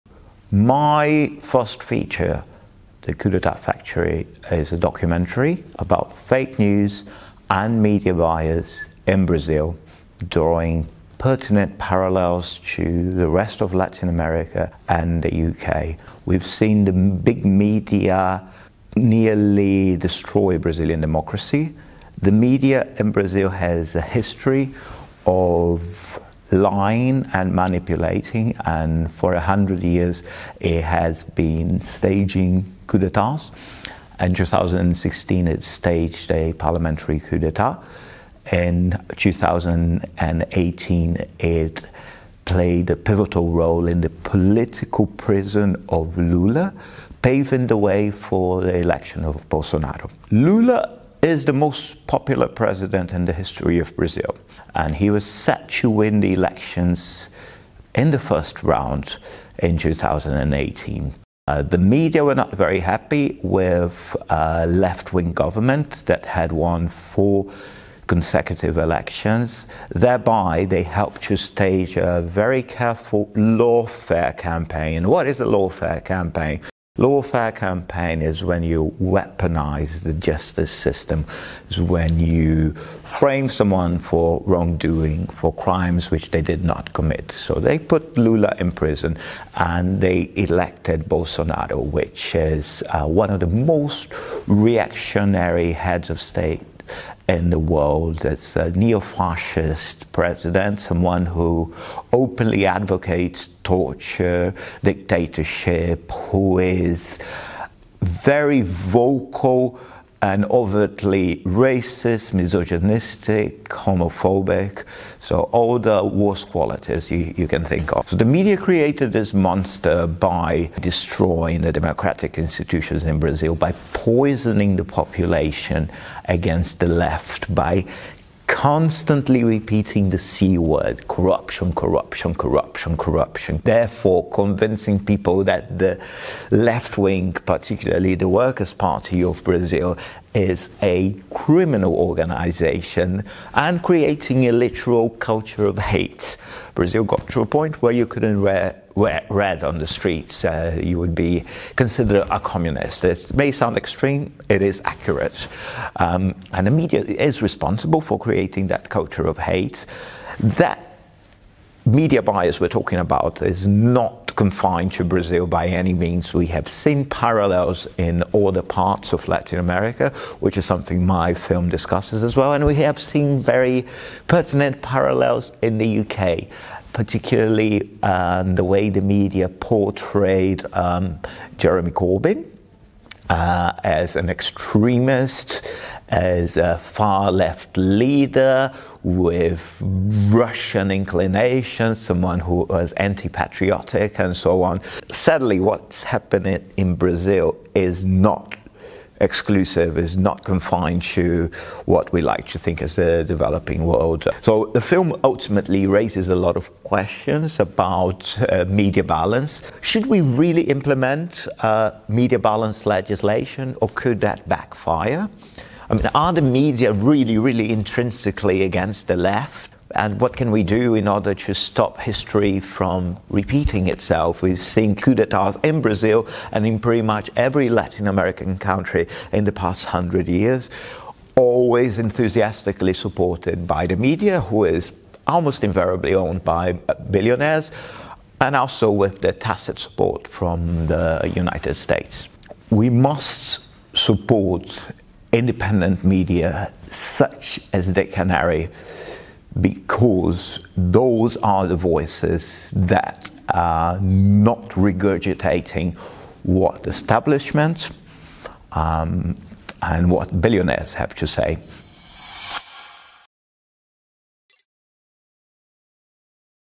From Brazil to the UK, corporate media is a threat to democracy
Filmmaker Victor Fraga has documented the way the corporate media in Brazil undermined the democratic process by falsely attacking the left and paving the way for the far right. Much the same process can be seen in the UK.

The UK premiere of Fraga's documentary, The Coup d´État Factory, takes place on Sunday 29 May at the BFI in London.